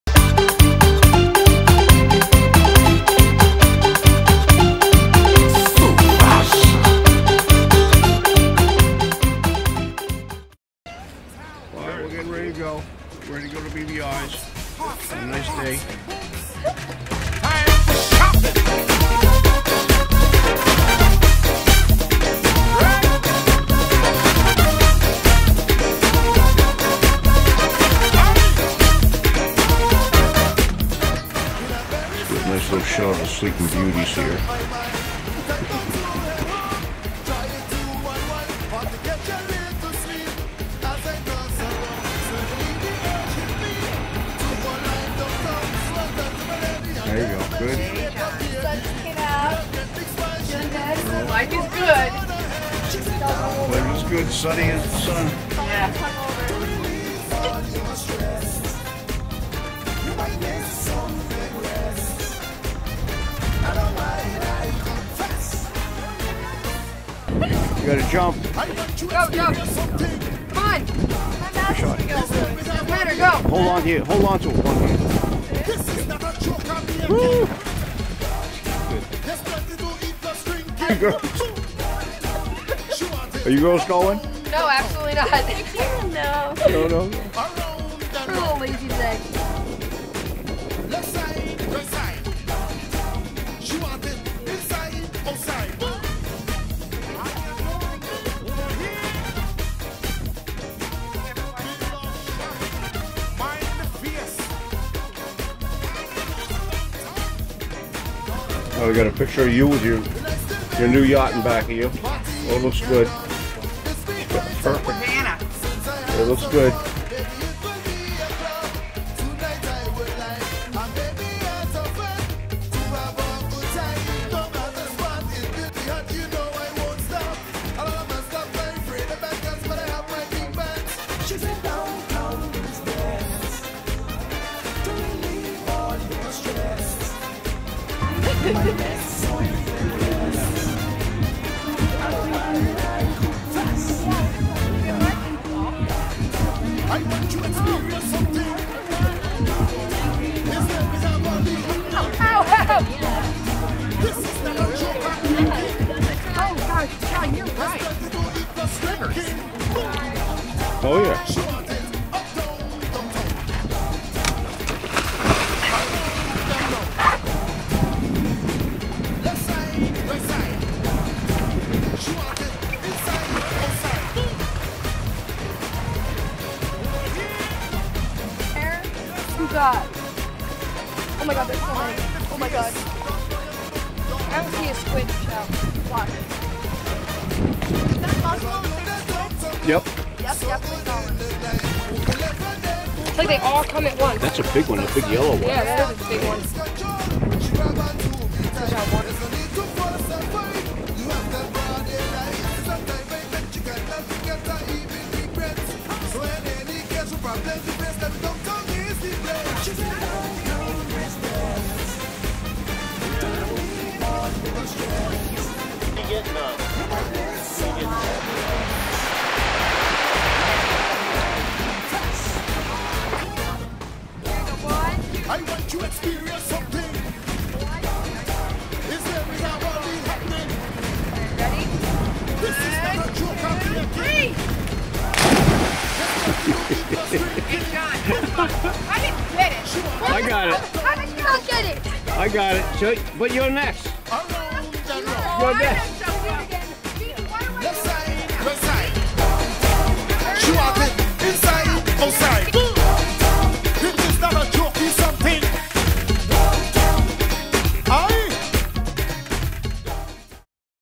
The Markees go on a sea cruise With Palm Tree Charters from St John USVI Virgin Islands
Palm Tree Charters takes a Sea Cruise from St John VI USVI to The Indians , The Caves and Pirates Bight on Norman Island,  and The Soggy Dollar Bar on Jost Van Dyke in the  B VI Virgin Islands